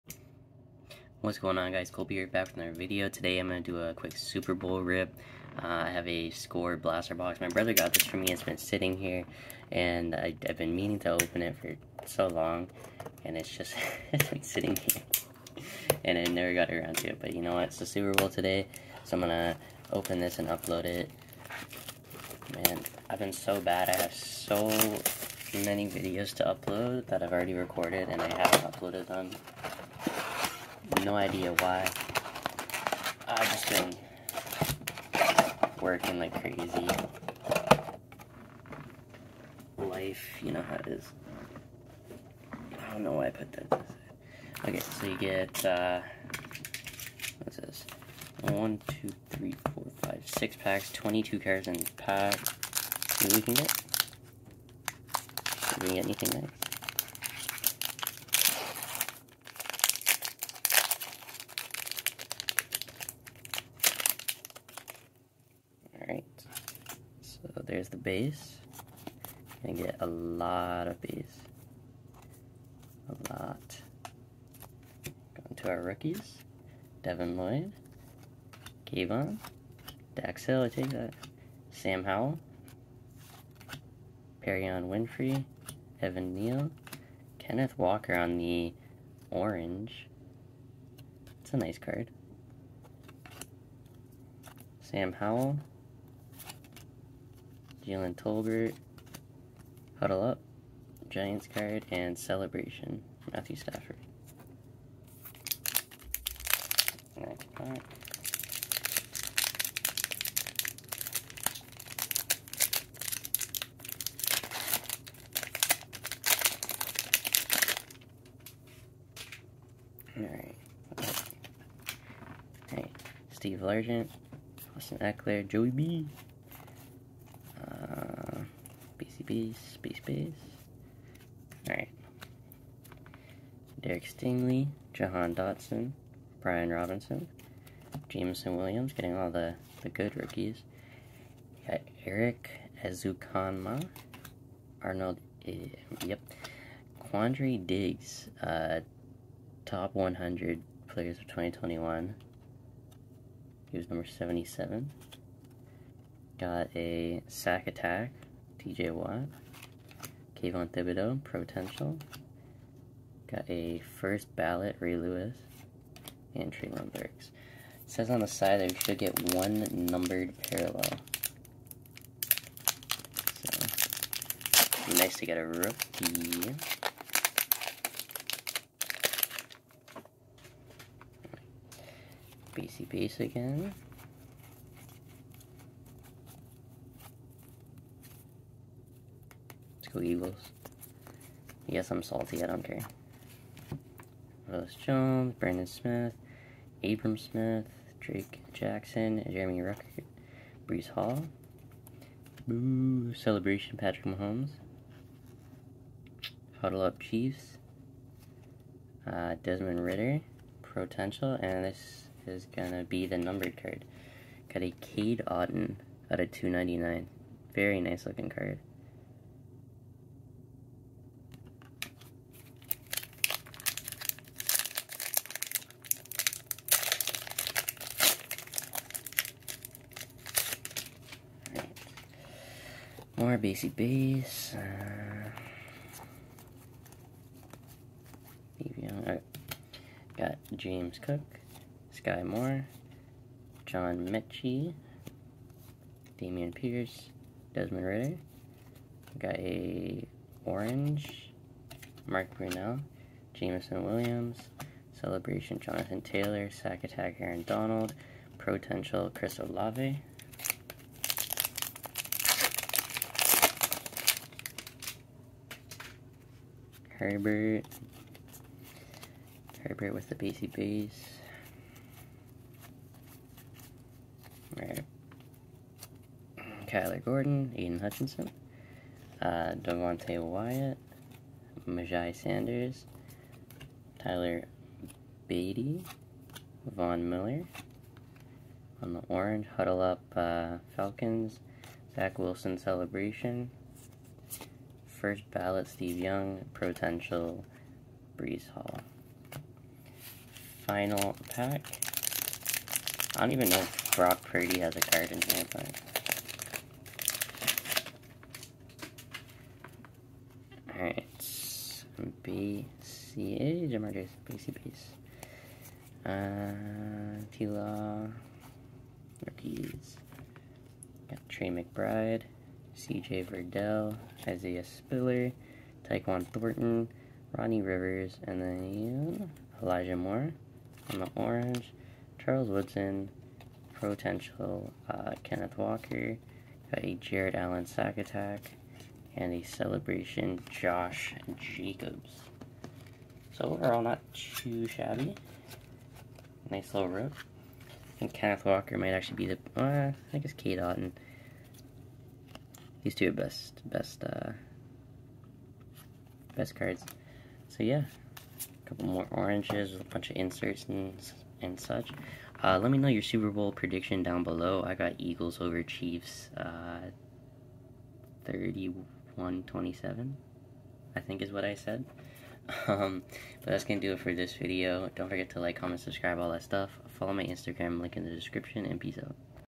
2022 Panini Score Football Blaster Box Opening! Super Bowl Prediction!
Hey guys back with a little super bowl rip. Fun lower end product here with Score. Solid pulls. Bummed that my Bengals aren't playing today but it's the freaking Super Bowl it's hard to not be excited as a football fan.

Let me know what you think about the pulls as well as your super bowl predictions.